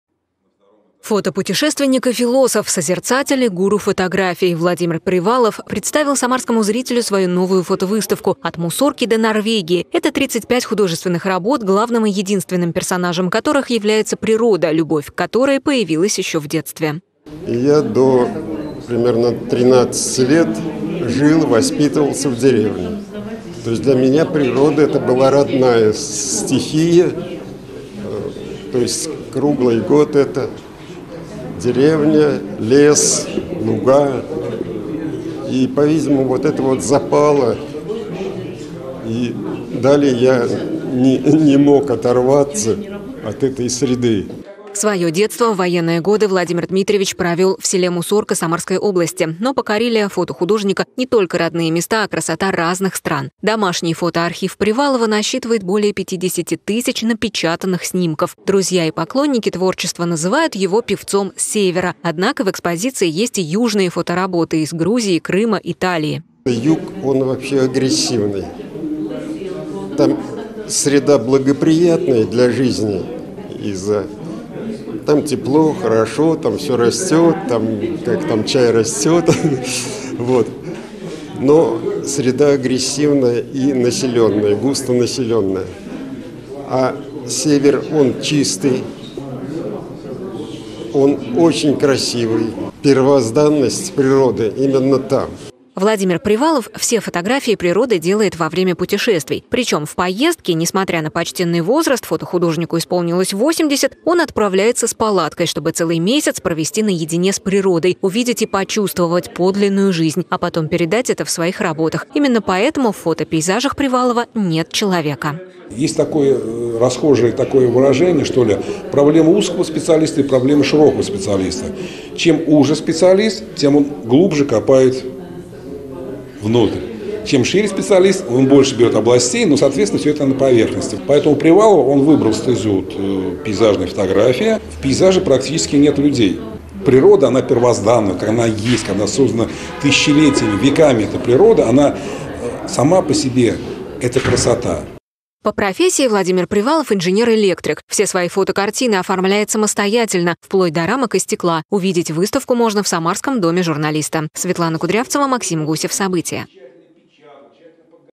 В Доме журналиста открылась выставка "От Мусорки до Норвегии"
Болота Вологодской области, заводи под Самарой, суровые северные фьорды и тепло южных полей. В Доме журналиста открылась выставка самарского фотографа Владимира Привалова "От Мусорки до Норвегии". В экспозиции 35 художественных работ, главным и единственным персонажем которых является природа. С подробностями Светлана Кудрявцева.